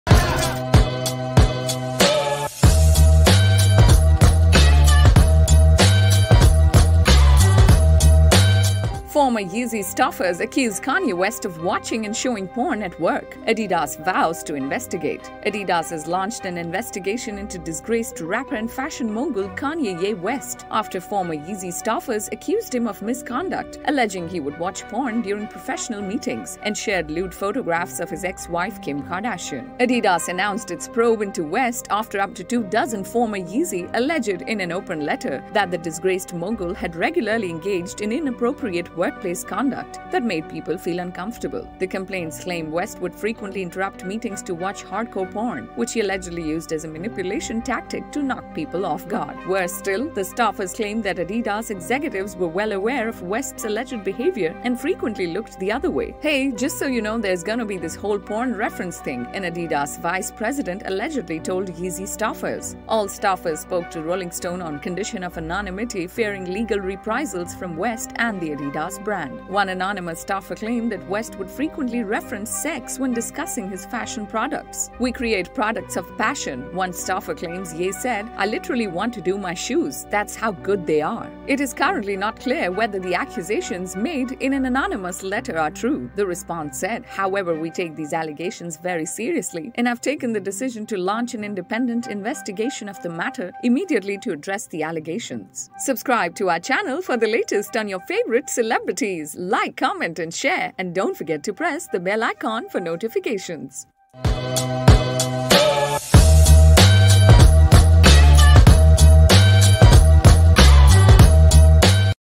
Adidas to investigate complaints Kanye West showed Kim Kardashian's explicit to employees
Adidas has launched an investigation into disgraced rapper and fashion mogul Kanye “Ye” West after former Yeezy